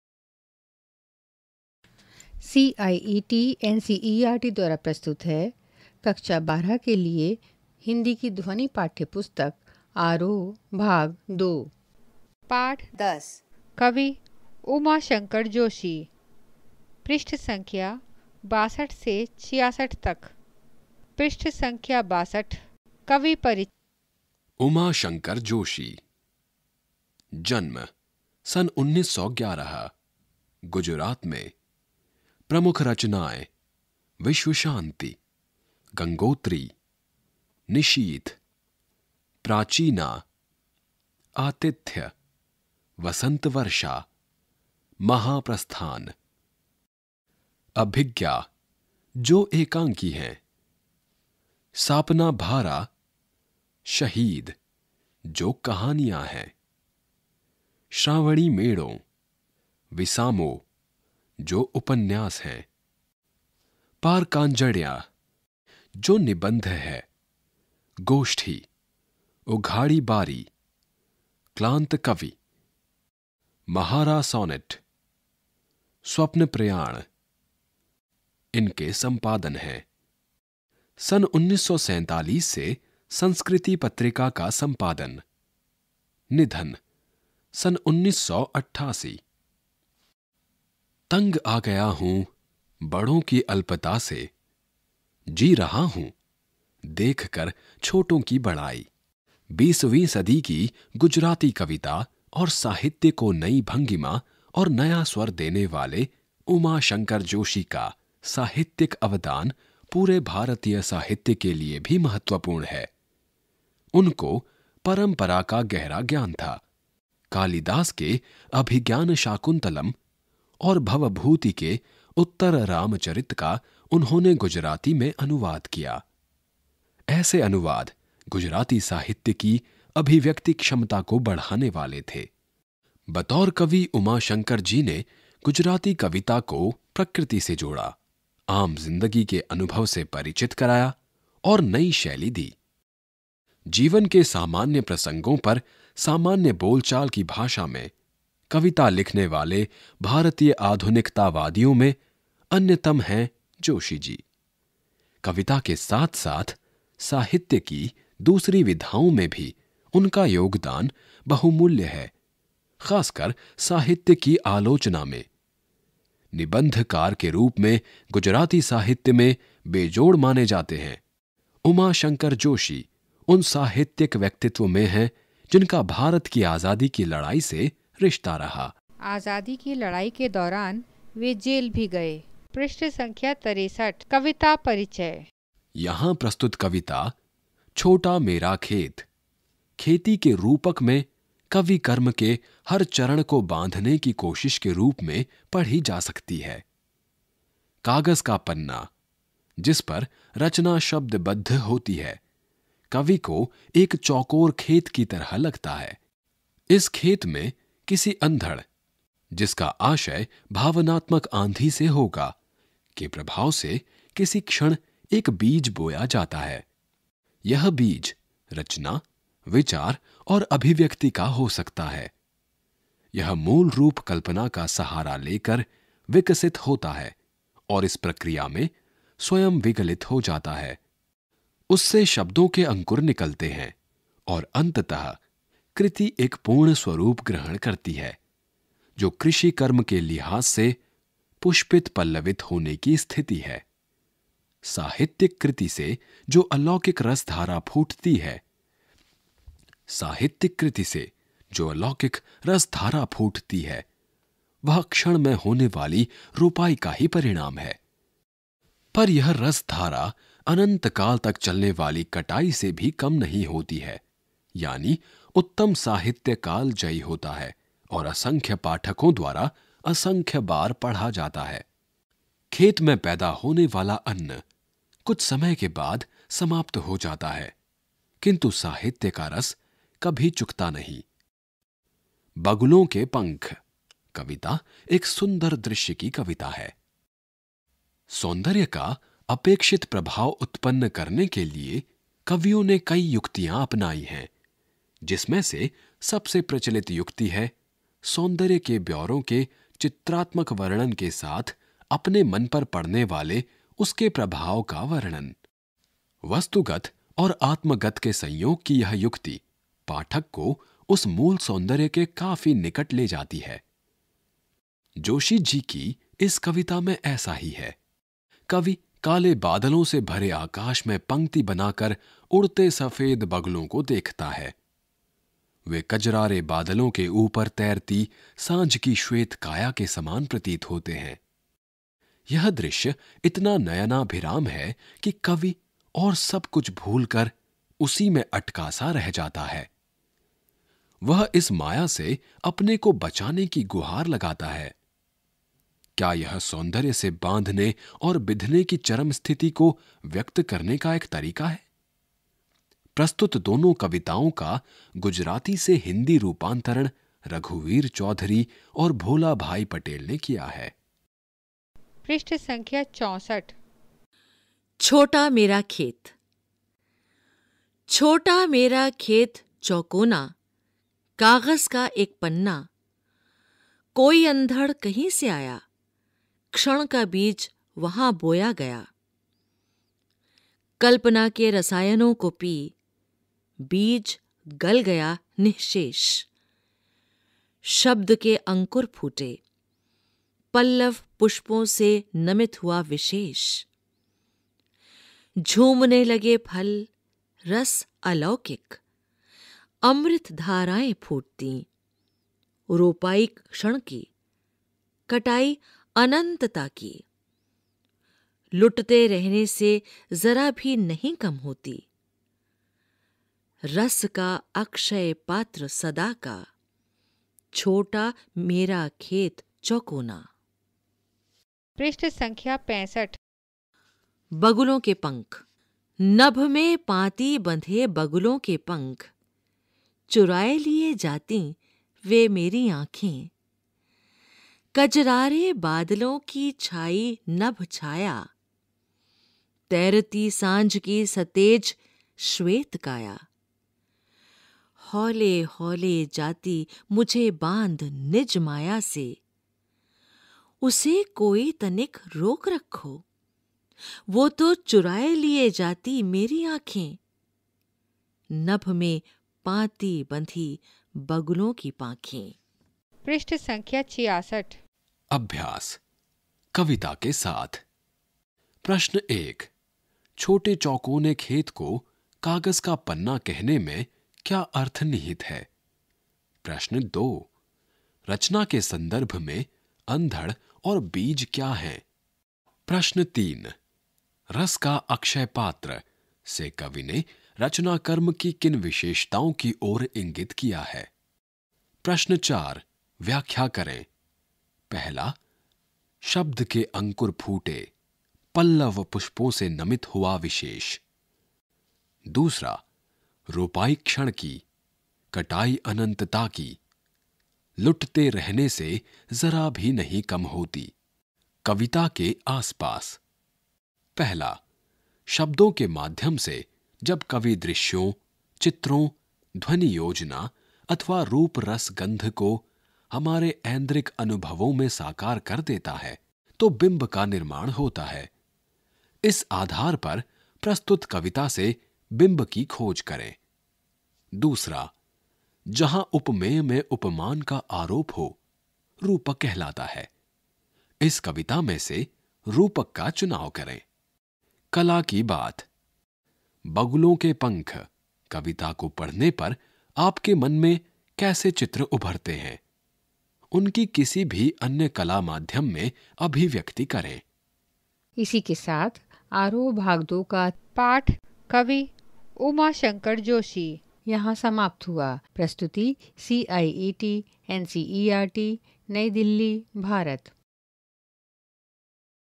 Chapter 10 Uma shanlkar Joshi Poet/पाठ-10 उमा शंकर
हिन्दी की यह ध्वनि पुस्तक कक्षा 12 के लिए है This is an audio book in Hindi for class 12